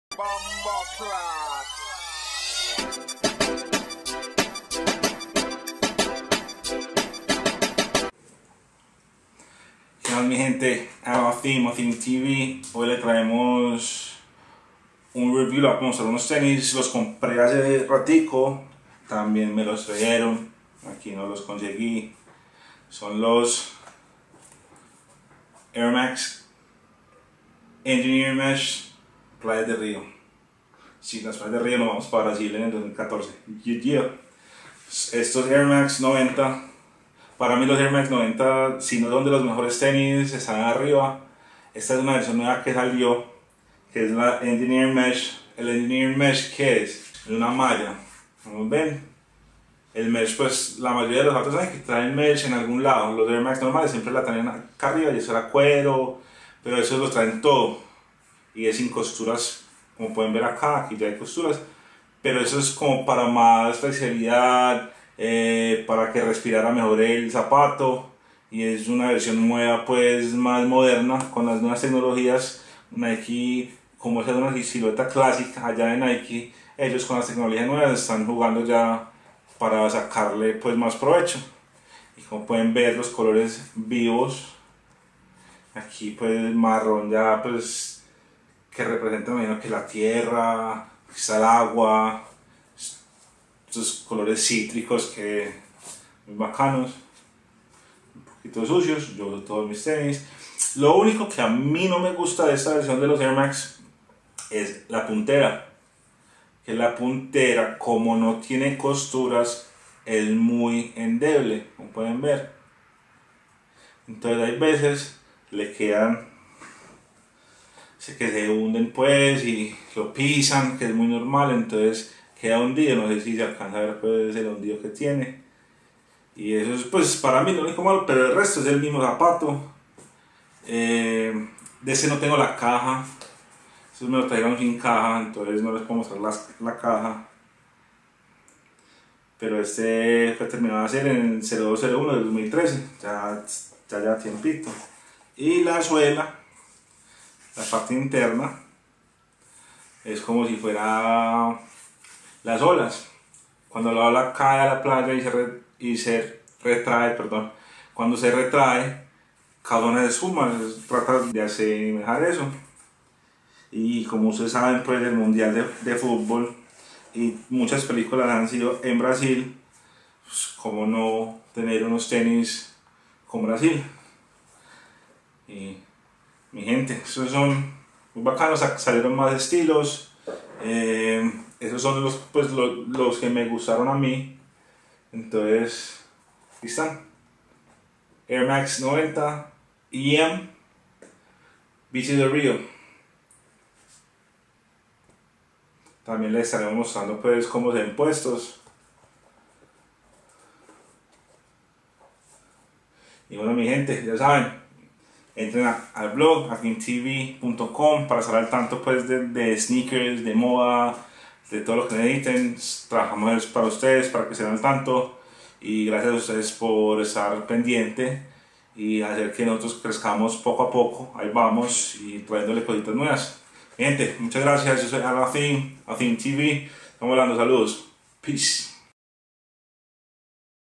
Nike Air Max 90 EM Beaches of Rio (Playas de Rio) reseña en español
Nike Air Max 90 EM Beaches of Rio (Playas de Rio). les traigo una adición a mi coleccione personal. los air max playas de rio son una versión mas moderna de la clásica silueta del air max 90. la malla usada lo deja respirar mas el pie  y lo hace un poco mas liviano.